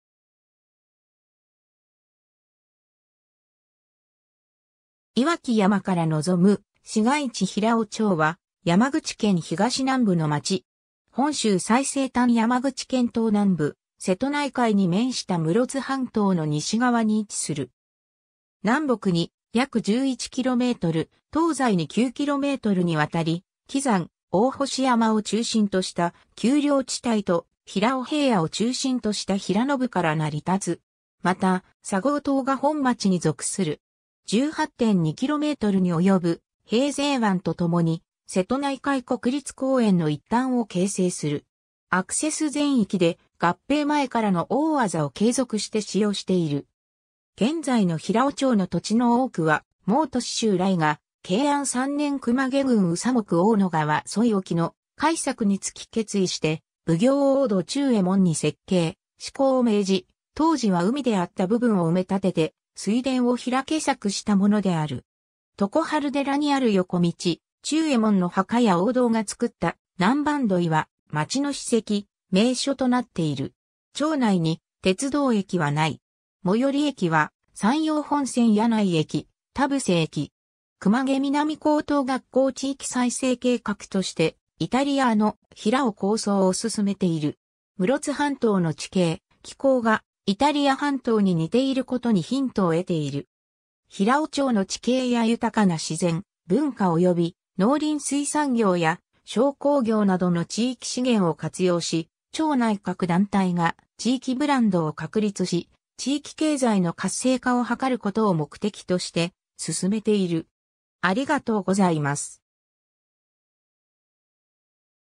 平生町
熊毛郡_(山口県)
石城山から望む市街地 平生町（ひらおちょう）は、山口県東南部の町。
 本州最西端山口県東南部、瀬戸内海に面した室津半島の西側に位置する。
南北に約11km、東西に9kmにわたり、箕山（標高400m）、大星山（標高438m）を中心とした丘陵地帯と、平生平野を中心とした平野部から成り立つ。
また、佐合島が本町に属する。
18.2kmにおよぶ平生湾と共に、瀬戸内海国立公園の一端を形成する。
 アクセス（車） 全域で、合併前からの大字を継続して使用している(但し曽根のみ合併時の新設)。
 現在の平生町の土地の多くは、毛利就頼が慶安三年（1650年）熊毛郡宇佐木大野河添い沖の開作につき決意して奉行横道忠右衛門に設計、施工を命じ、当時は海であった部分を埋め立てて水田を開作したものである。
 常春寺にある横道忠右衛門の墓や横道が造った南蛮樋（水門）は、町の史跡・名所となっている。
 町内に鉄道駅は無い。
最寄り駅は山陽本線柳井駅・田布施駅。
 熊毛南高等学校 地域再生計画として、「イタリアーノひらお」構想を進めている。
室津半島の地形、気候がイタリア半島に似ていることにヒントを得ている。
平生町の地形や豊かな自然、文化及び農林水産業や商工業などの地域資源を活用し、町内各団体が地域ブランドを確立し地域経済の活性化を図ることを目的として進めている。